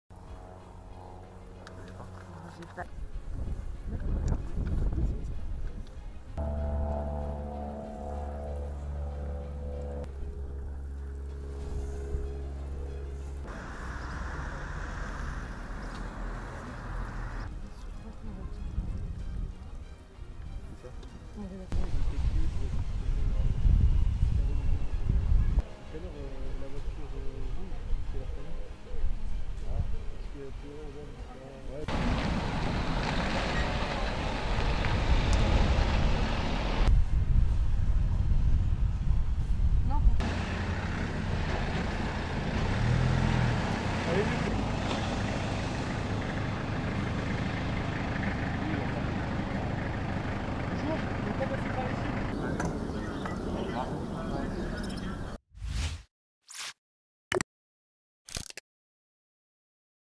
4 قتلى بتحطم طائرة سياحية صغيرة قرب باريس
قتل 4 أشخاص، بحادث تحطم طائرة سياحية صغيرة على بعد 40 كيلومتراً شمال شرقي العاصمة الفرنسية باريس، وعثر على حطام الطائرة التي أقلعت من بوفيه، في حقل بالقرب من سانت باتوس، في وقت بدأت فيه السلطات التحقيق لمعرفة ملابسات الحادث.